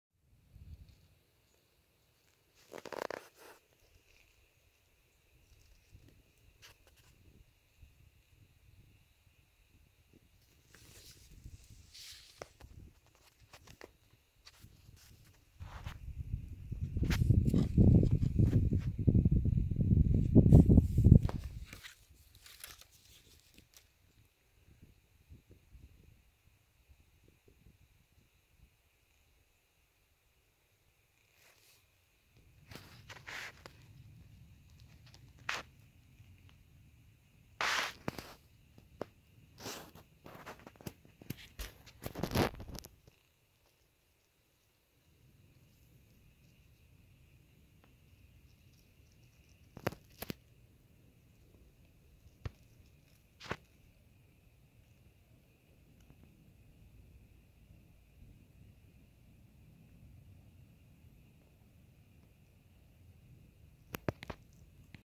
Sierra Nevada pine processionary caterpillar: Thaumetopoea pityocampa
The pine Processionary caterpillar has spines that will irritate skin tissue and can cause an allergic reaction. Later in its life cycle it will undergo metamorphosis and transform into a moth. The caterpillars leave a pheromone trail that allows others to follow. They can sometimes be found together travailing in a train.